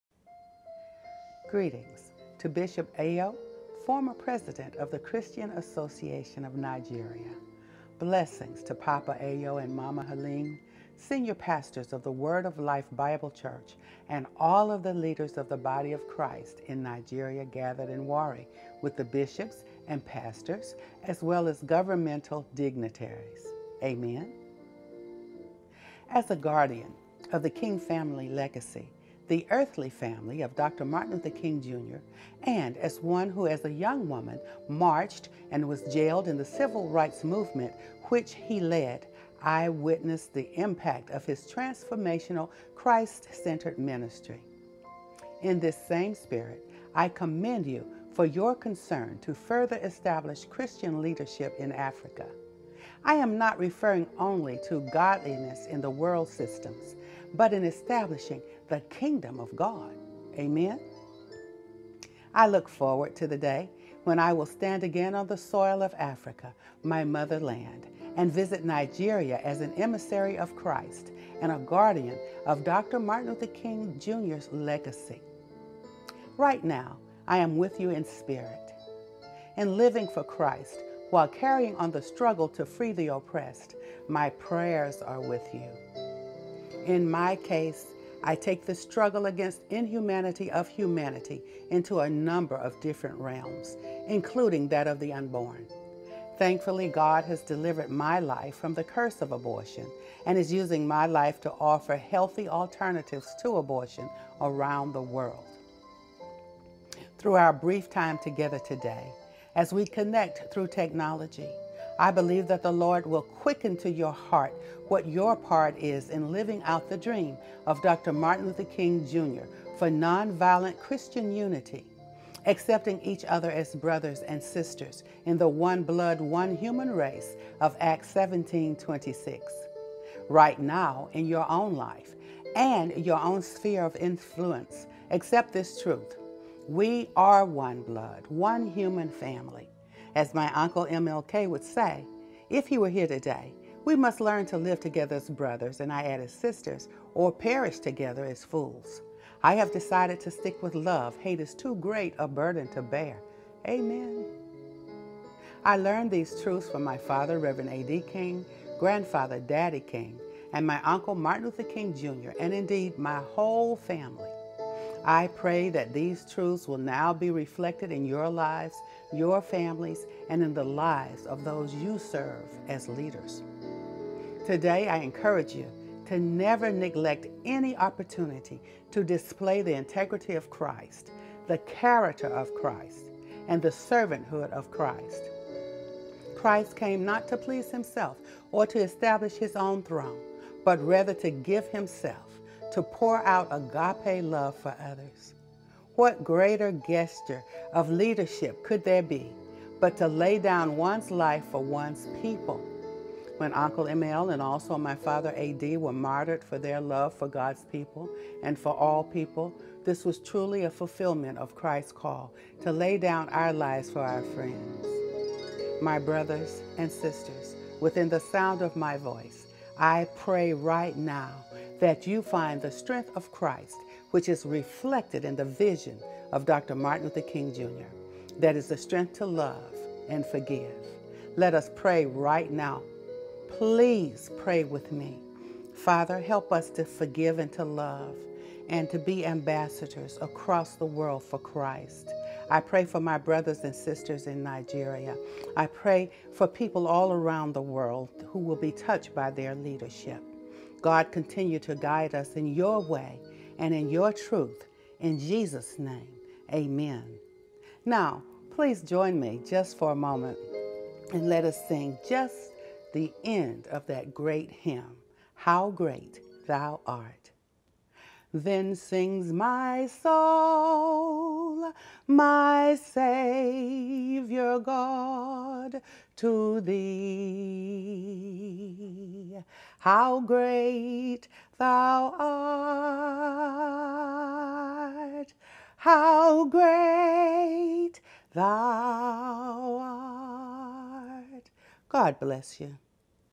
Nigerian Leadership Conference
Evangelist Alveda King - Greetings to the Nigerian Leadership Conference

Novemeber 15, 2017